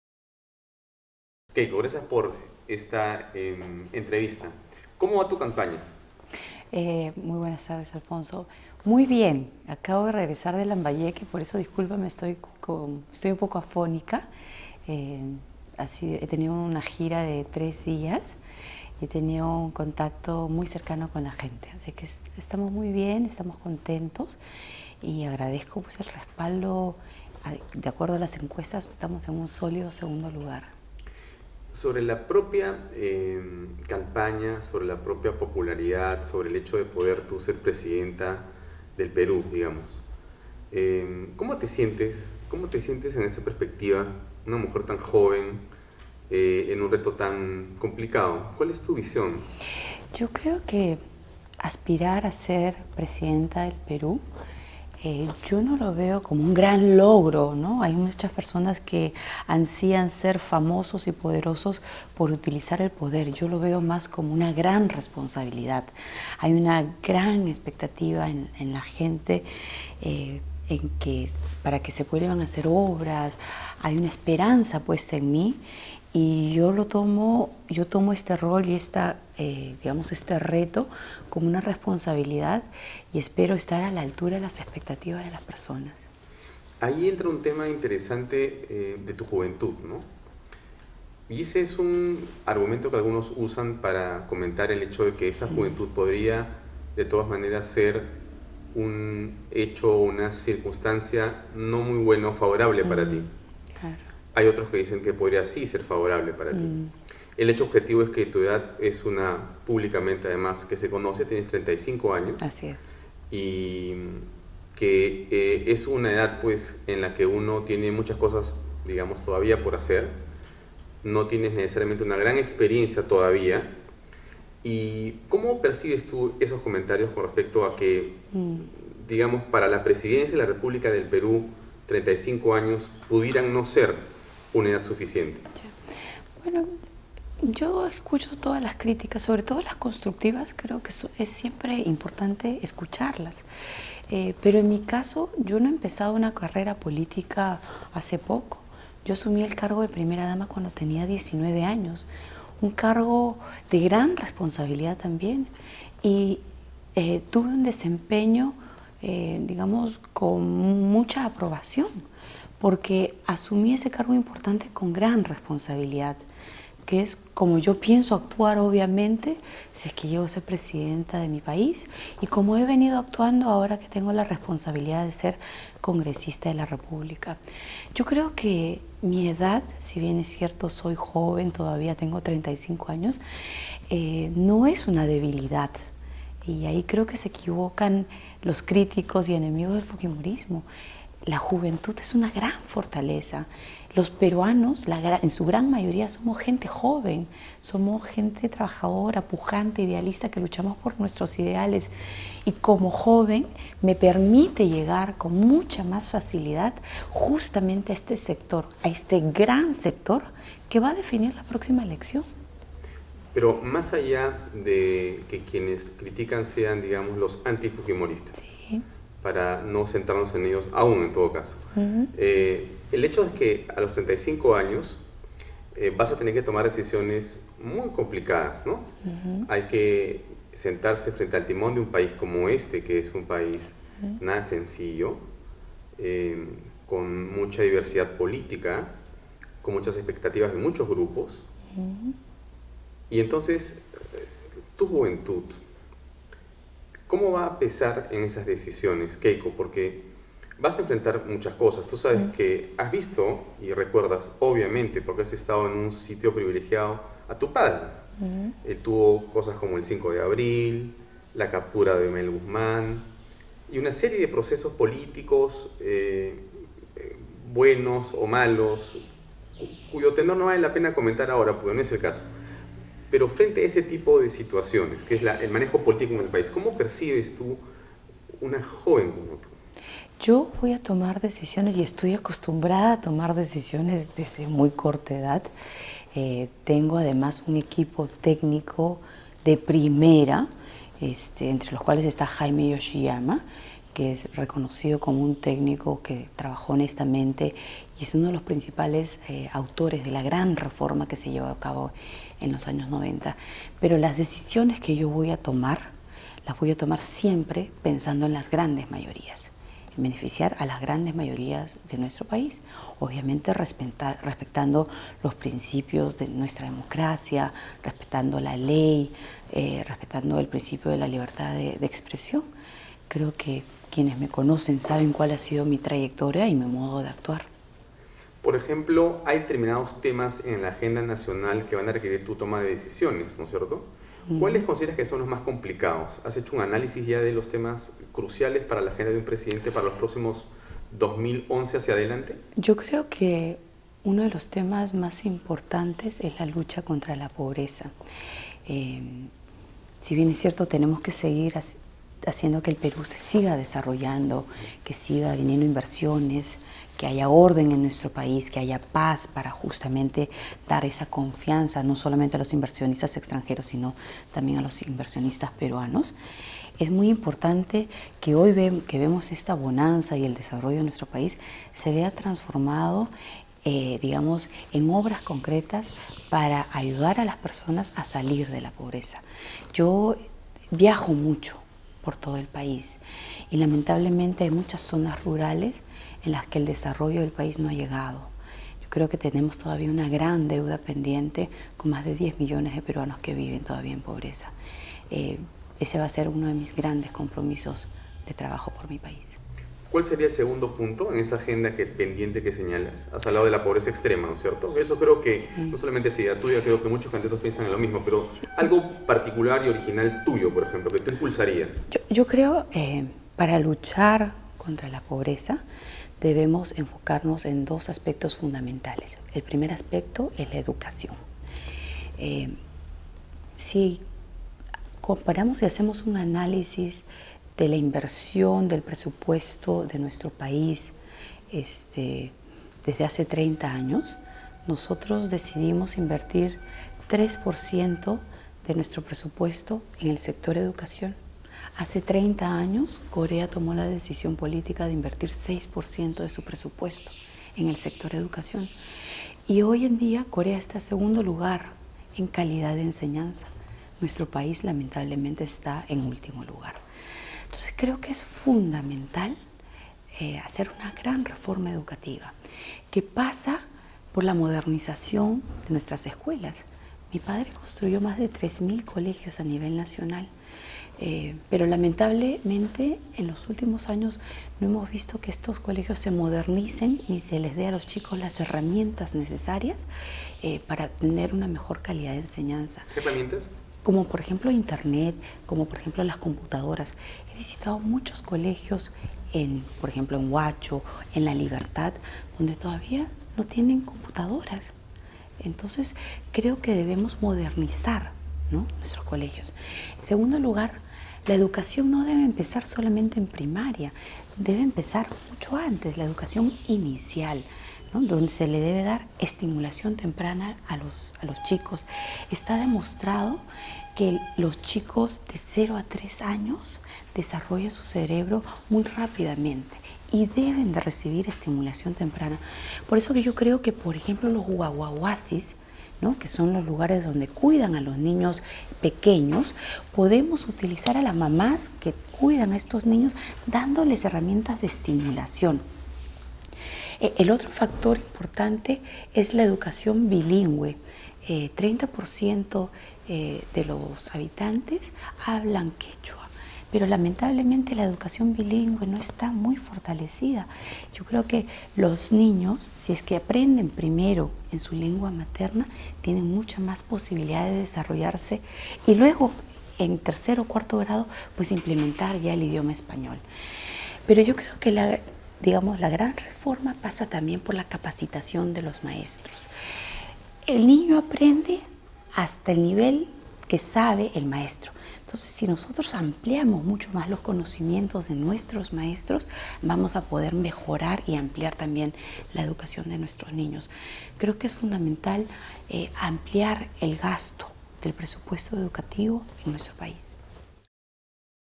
KEIKO FUJIMORI con Alfonso Baella Herrera, 1ra parte
Keiko Fujimori, Candidata a la Presidencia de la República por Fuerza 2011
Tema: Sobre su candidatura a la Presidencia de la República